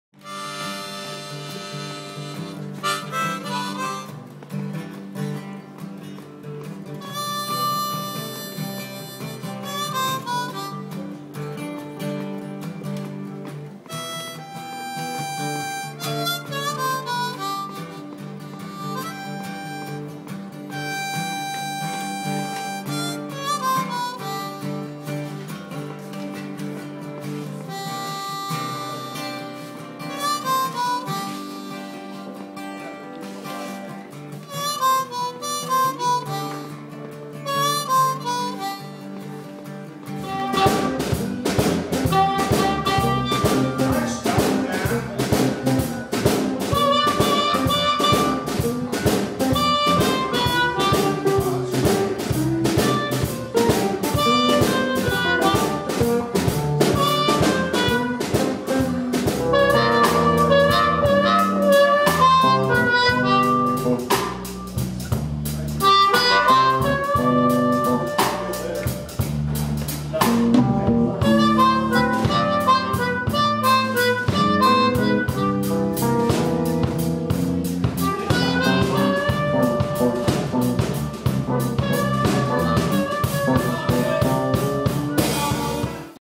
Kingfish Kirk at Blues Harmonica Jam Camp
Kingfish Kirk Hansen reels it in at Jon Gindick's Blues Harmonica Jam Camp in Clarksdale, Mississippi - March 2011.